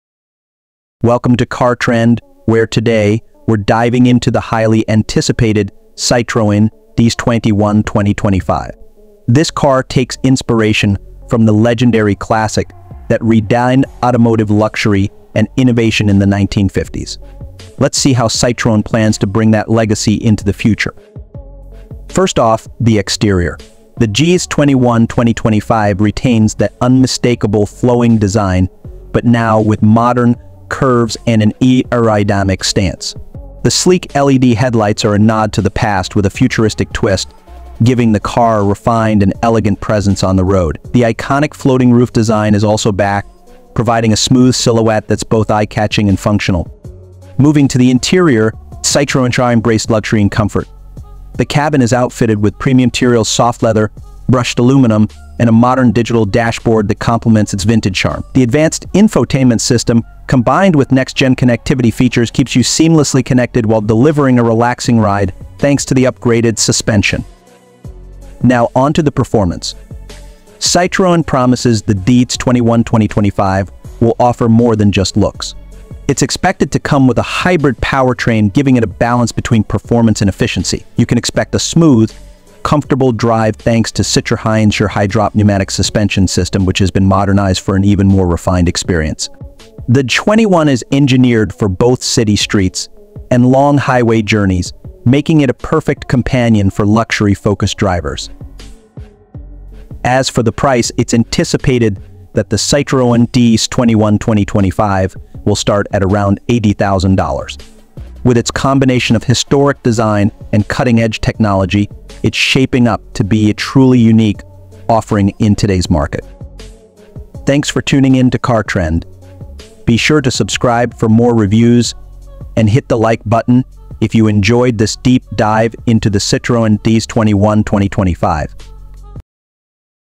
Citroën DS21 2025: The Iconic Classic Reborn with a Futuristic Twist!
Description:

The Citroën DS21 2025 is not just a car; it’s a bold return of one of the most iconic models in automotive history. Famed for its revolutionary design and technology when it first debuted in the 1950s, the DS21 became a symbol of French innovation and elegance. Now, in 2025, Citroën revives this classic with a modern twist, blending timeless design cues with cutting-edge technology, luxury, and performance. This new edition promises to captivate both fans of the original and a new generation of drivers looking for a blend of history and futuristic innovation.

Price:

The Citroën DS21 2025 is expected to have a starting price of approximately $80,000. This price reflects its luxurious features, innovative technology, and the modern twist on its iconic design, making it a premium choice in the market.

This Content is AI Generated: